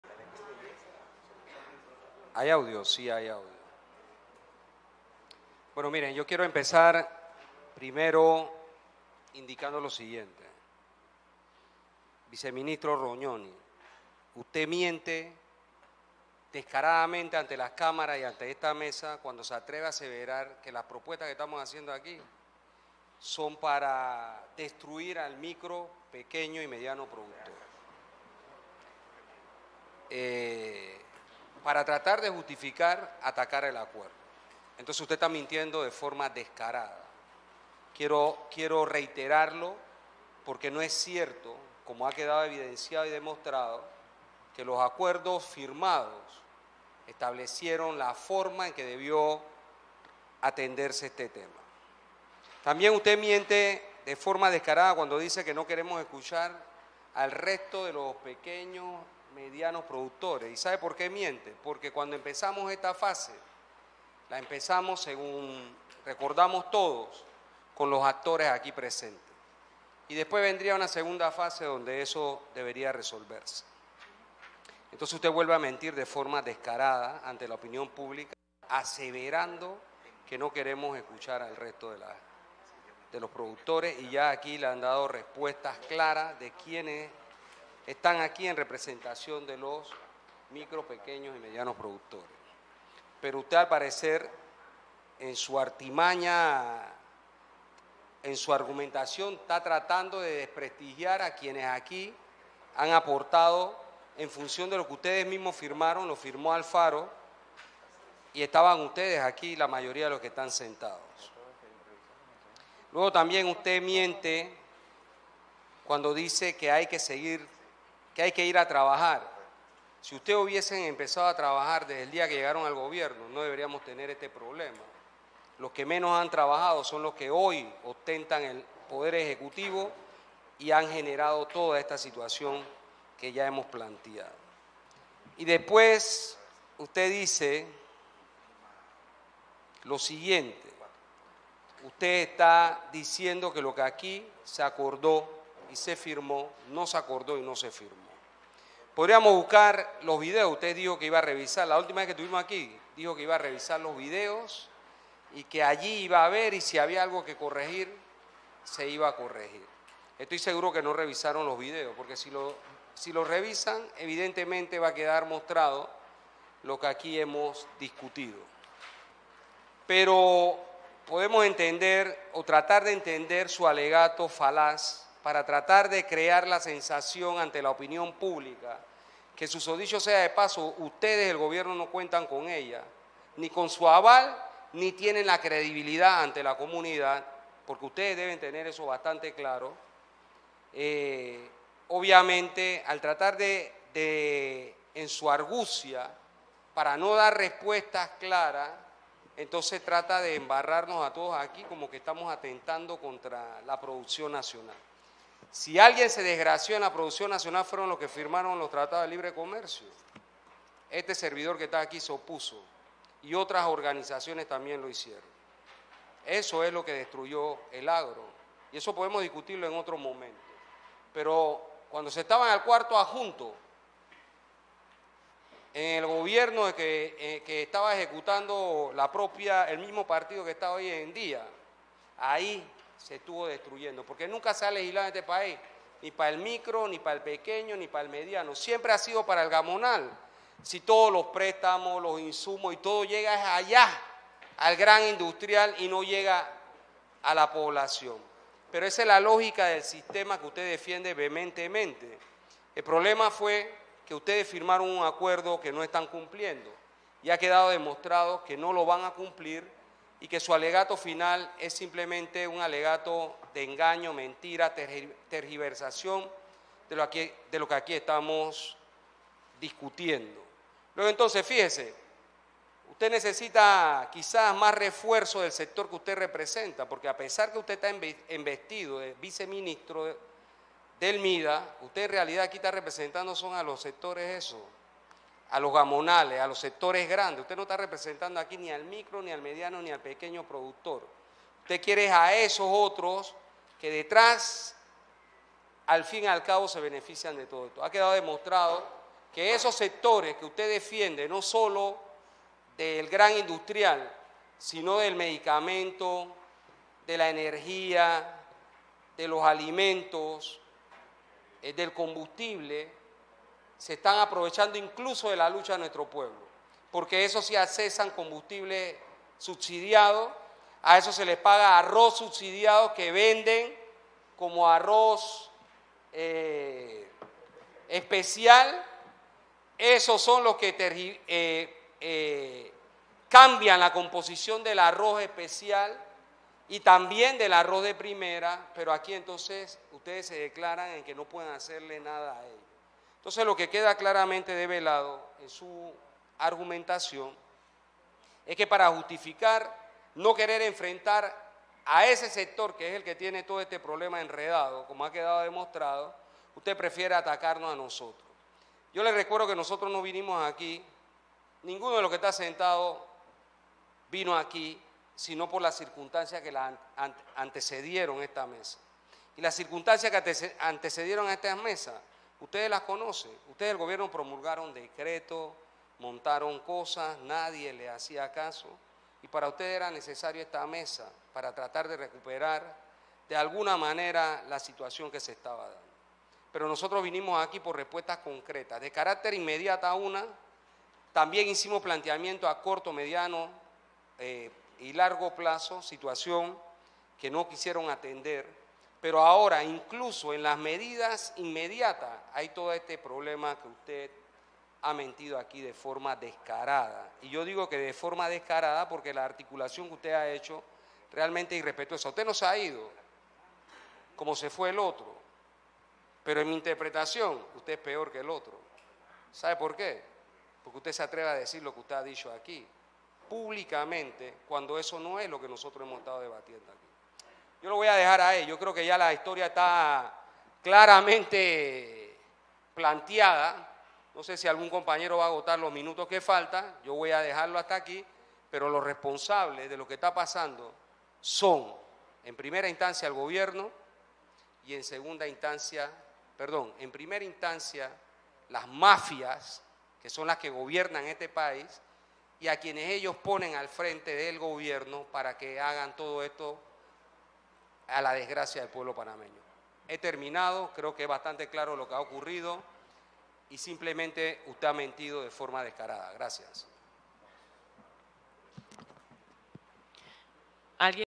Respuesta de Saúl Méndez al irrespeto del Vice-Ministro Carlo Rognoni
Alianza pueblo unido por la vida